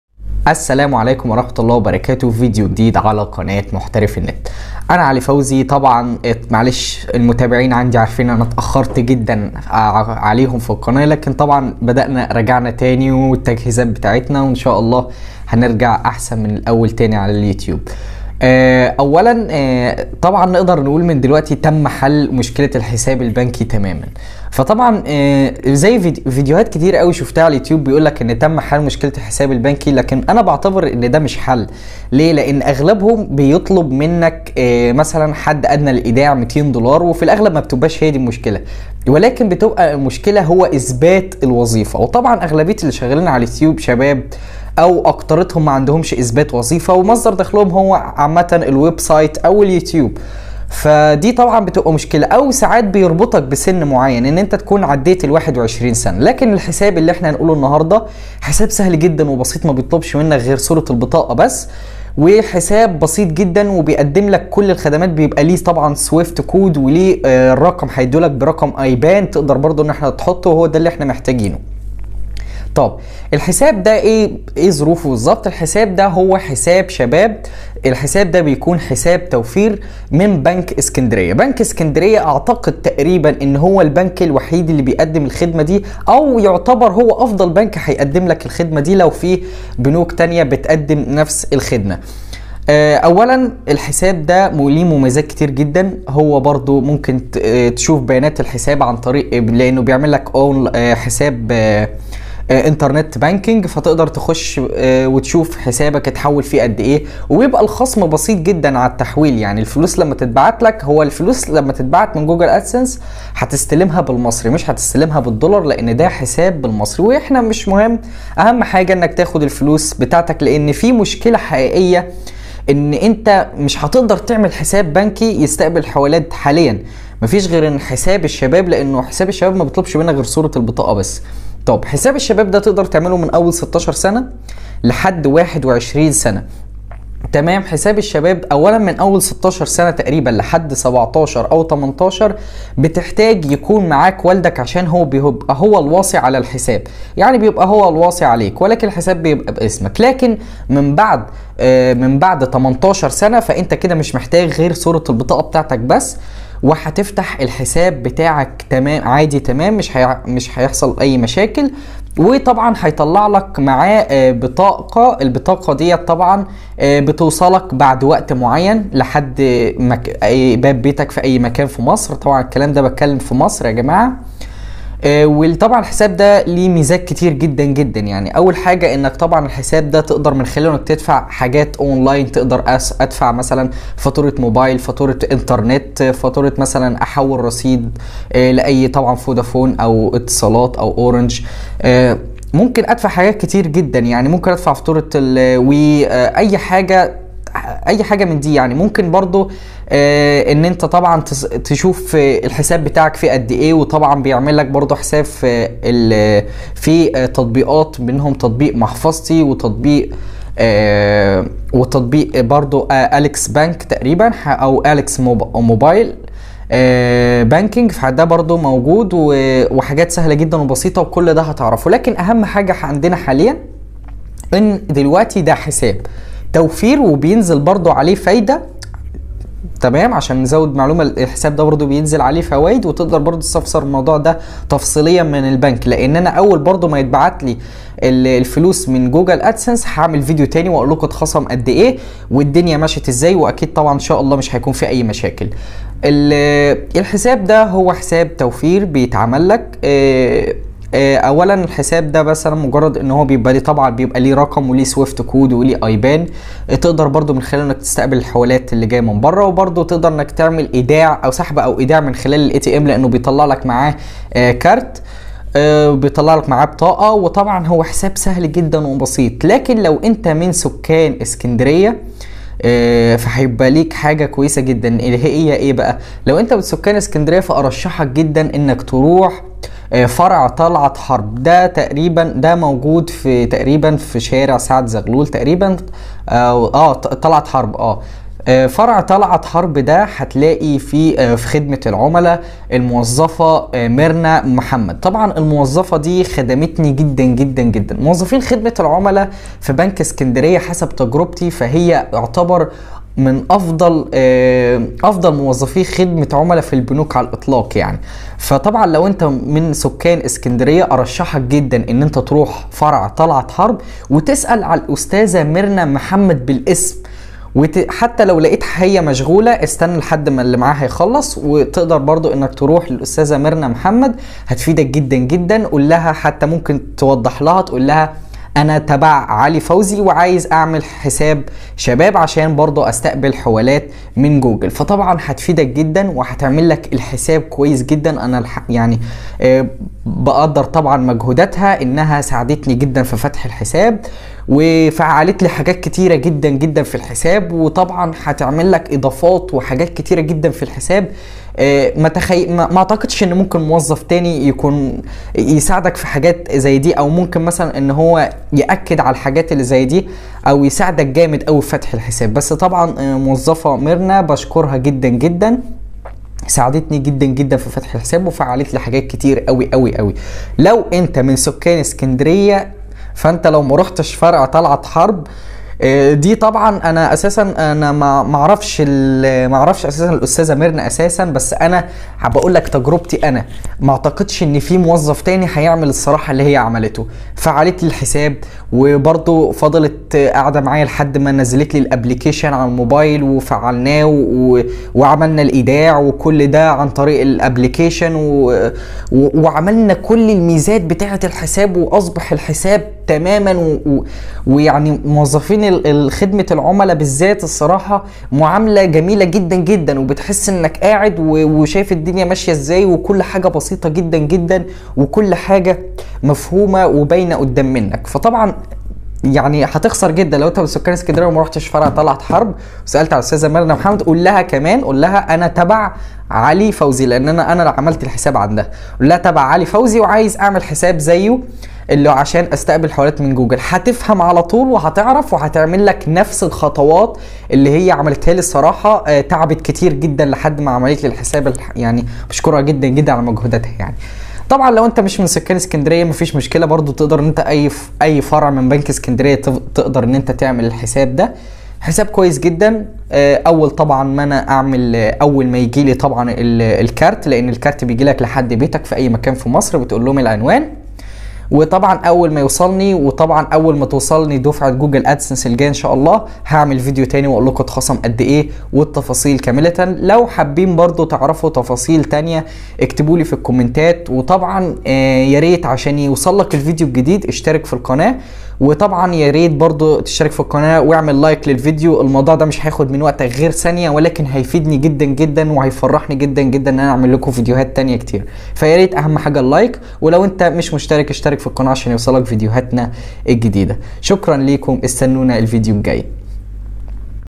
افضل حساب بنكي لاستلام ارباح ادسنس بالبطاقة فقط بدون اثبات دخل
في هذا الفيديو سوف نتعرف على افضل حساب بنكي لاستلام ارباح ادسنس بالبطاقة فقط بدون اثبات دخل وهو اسهل حساب بنكي لاستلام ارباح اليوتيوب او ارباح جوجل ادسنس بدون اثبات دخل او بدون سن معين ويمكنك من خلال هذا الحساب استلام الارباح او استلام دفعات ادسنس على الحساب البنكي بدون مشاكل

عنوان فرع بنك الاسكندرية في محافظة الاسكندرية
21 طلعت حرب، المسلة غرب وشريف باشا، العطارين، الإسكندرية